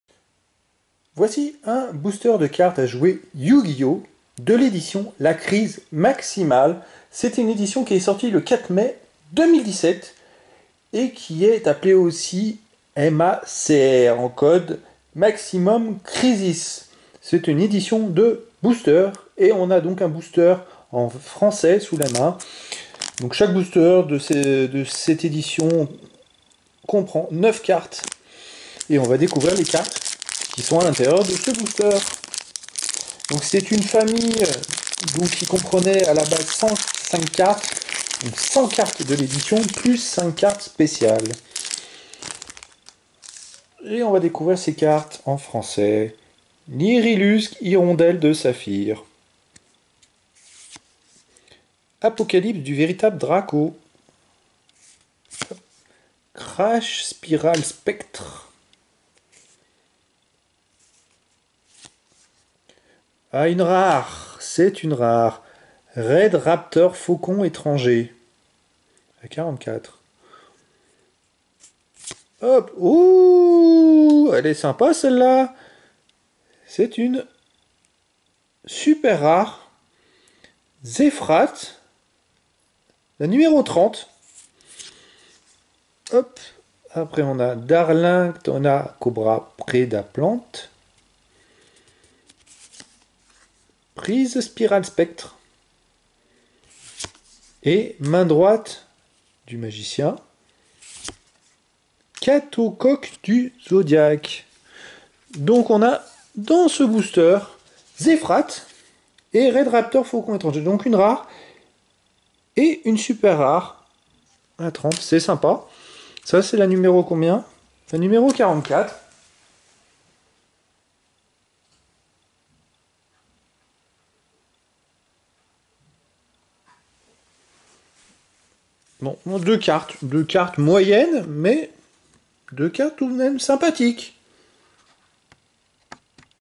Ouverture d'un booster la Crise Maximale MACR, Maximum Crisis, cartes
Cartes Yu Gi Oh - MACR ! Ouverture d'un booster la Crise Maximale ! Sortie le 04 mai 2017 !

Unboxing Yu-Gi-Oh Booster Cards Maximum Crisis !

Yu-Gi-Oh! Trading Card Game ! Konami !